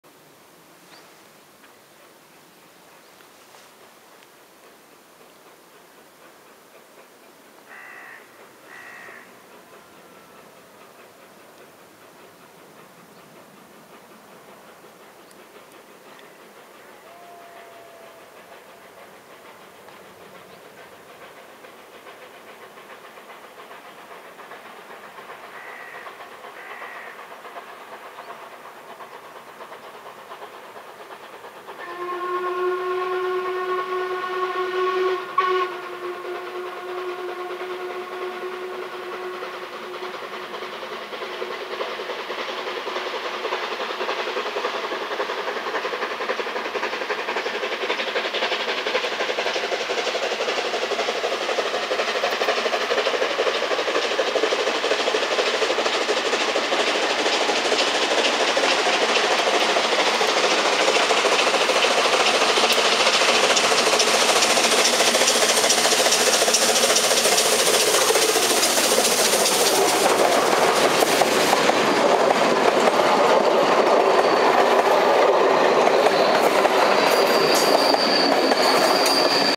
Papagáj - rozjazd zo zástavky Dedinky
Prejazd parného rušňa pri Dedinkách, Palcmanskou mašou.